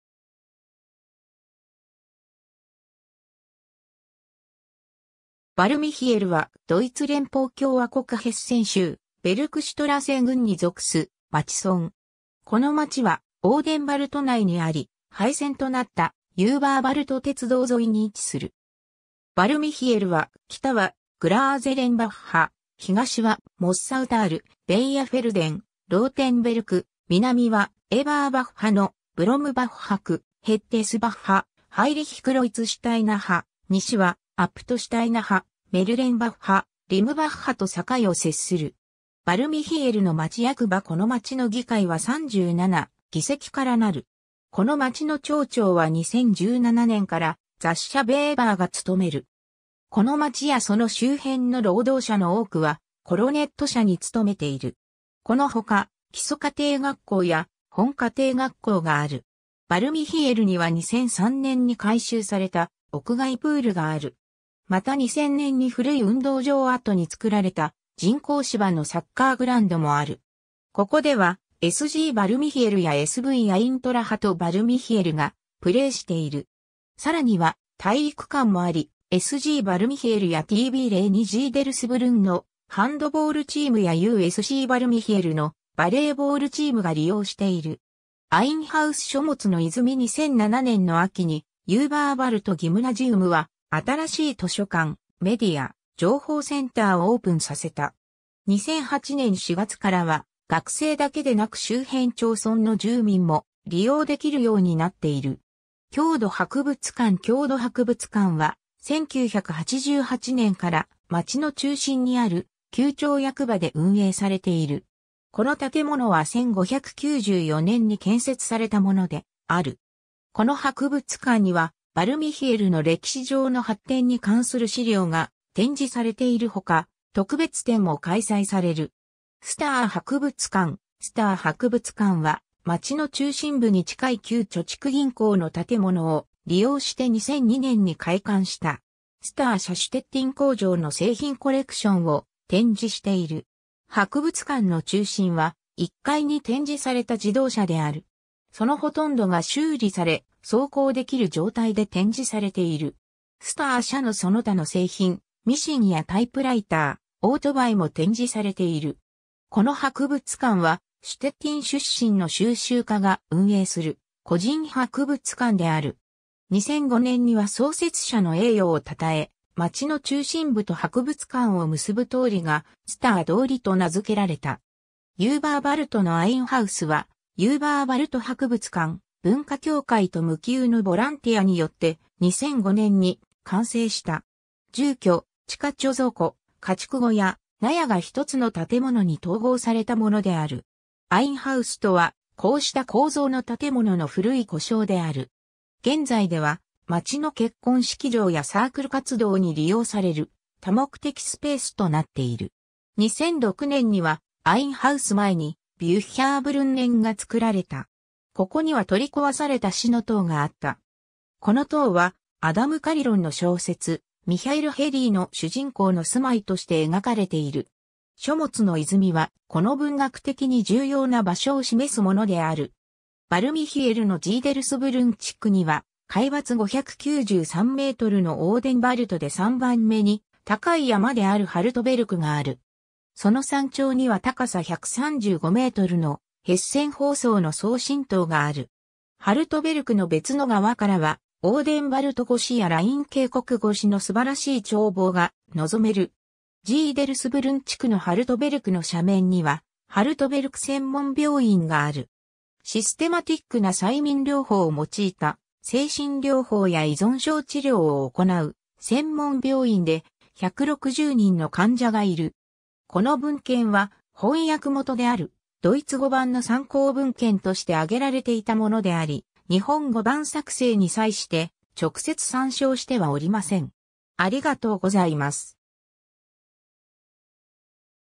ヴァルト＝ミヒェルバッハ
ヴァルト＝ミヒェルバッハ (ドイツ語: Wald-Michelbach) はドイツ連邦共和国ヘッセン州ベルクシュトラーセ郡に属す町村（以下、本項では便宜上「町」と記載する）。
この町はオーデンヴァルト内にあり、廃線となったユーバーヴァルト鉄道沿いに位置する。
 ヴァルト＝ミヒェルバッハは、北はグラーゼレンバッハ、東はモッサウタール、ベーアフェルデン、ローテンベルク（東の3市町村はオーデンヴァルト郡）、南はエーバーバッハのブロムバッハ区、ヘッデスバッハ、ハイリヒクロイツシュタイナハ（南の3市町村はいずれもバーデン＝ヴュルテンベルク州ライン＝ネッカー郡）、西はアプトシュタイナハ、メルレンバッハ、リムバッハと境を接する。
 ヴァルト＝ミヒェルバッハの町役場 この町の議会は37議席からなる。
 この町の町長は2017年からザッシャ・ヴェーバー (SPD) が務める。
 この町やその周辺の労働者の多くはコロネット社に務めている。
 この他、基礎課程学校や本課程学校がある。
 ヴァルト＝ミヒェルバッハには2003年に改修された屋外プールがある。
また2000年に古い運動場跡に造られた人工芝のサッカーグランドもある。
ここではSGヴァルト＝ミヒェルバッハやSVアイントラハト・ヴァルト＝ミヒェルバッハがプレイしている。
さらには体育館もあり、SGヴァルト＝ミヒェルバッハやTV02ジーデルスブルンのハンドボールチームやÜSCヴァルト＝ミヒェルバッハのバレーボールチームが利用している。
 アインハウス 書物の泉 2007年の秋にユーバーヴァルト・ギムナジウムは新しい図書館メディア・情報センターをオープンさせた。
2008年4月からは学生だけでなく周辺町村の住民も利用できるようになっている。
 郷土博物館 郷土博物館は、1988年から町の中心にある旧町役場で運営されている。
この建物は1594年に建設されたものである。
この博物館にはヴァルト＝ミヒェルバッハの歴史上の発展に関する資料が展示されているほか、特別展も開催される。
 Stoewer博物館 Stoewer博物館は、町の中心部に近い旧貯蓄銀行の建物を利用して2002年に開館した。
Stoewer社シュテッティン工場（en:Stoewer参照）の製品コレクションを展示している。
博物館の中心は、1階に展示された自動車である。
そのほとんどが修理され、走行できる状態で展示されている。
Stoewer社のその他の製品、ミシンやタイプライター、オートバイも展示されている。
この博物館はシュテッティン出身の収集家が運営する個人博物館である。
2005年には創設者の栄誉を称え、町の中心部と博物館を結ぶ通りが「Stoewer通り」と名付けられた。
 ユーバーヴァルトのアインハウスはユーバーヴァルト博物館・文化協会と無給のボランティアによって、2005年に完成した。
住居、地下貯蔵庫、家畜小屋、納屋が一つの建物に統合されたものである。
アインハウスとはこうした構造の建物の古い呼称である。
現在では町の結婚式場やサークル活動に利用される多目的スペースとなっている。
2006年にはアインハウス前にビュッヒャーブルンネン（書物の泉）が造られた。
ここには取り壊された市の塔があった。
この塔はアダム・カリロンの小説『ミヒャエル・ヘリー』の主人公の住まいとして描かれている。
書物の泉は、この文学的に重要な場所を示すものである。
 ヴァルト＝ミヒェルバッハのジーデルスブルン地区には海抜593mのオーデンヴァルトで3番目に高い山であるハルトベルクがある。
その山頂には高さ135mのヘッセン放送の送信塔がある。
ハルトベルクの別の側からはオーデンヴァルト越しやライン渓谷越しの素晴らしい眺望が望める。
 ジーデルスブルン地区のハルトベルクの斜面にはハルトベルク専門病院がある。
システマティックな催眠療法を用いた精神療法や依存症治療を行う専門病院で、160人の患者がいる。
 この文献は、翻訳元であるドイツ語版の参考文献として挙げられていたものであり、日本語版作成に際して直接参照してはおりません。